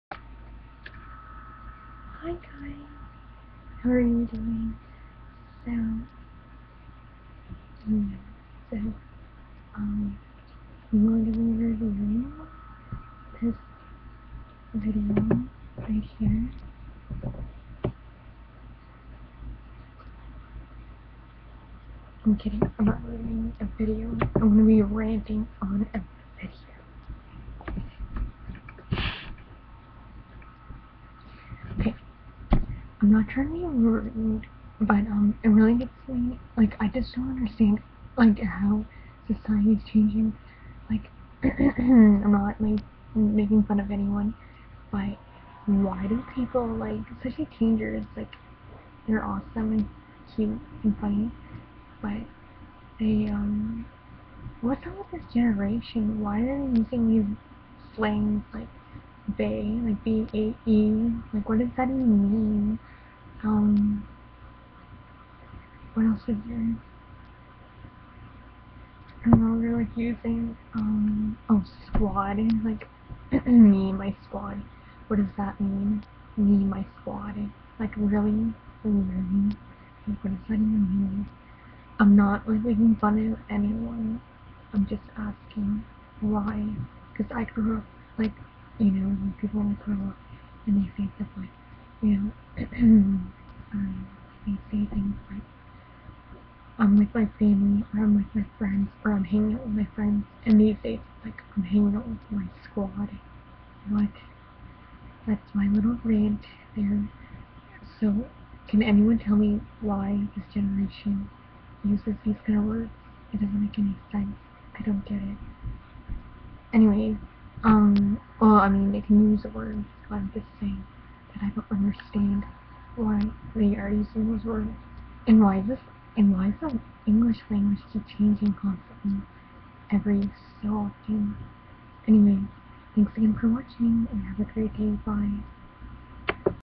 TEENS + Words = ?
teens using new words makes Miss Jessica Kardashian Confused but I still love the new words but so many words in the English language lol already.